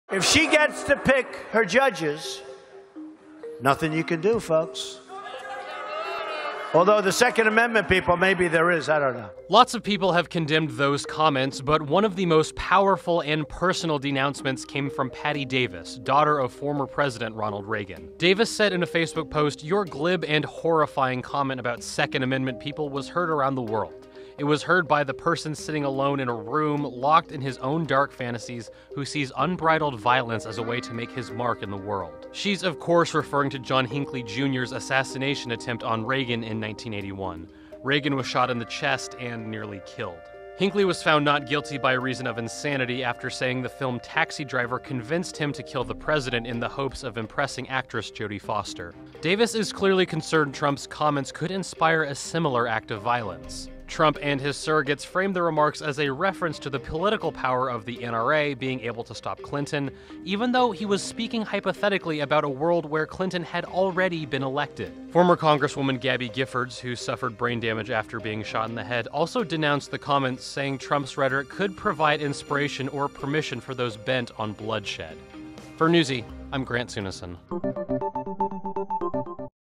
Reagan's daughter denounces Trump's comments
Ronald Reagan's daughter Patti Davis warned that Donald Trump's Second Amendment comments could incite violence and that he should watch his rhetoric.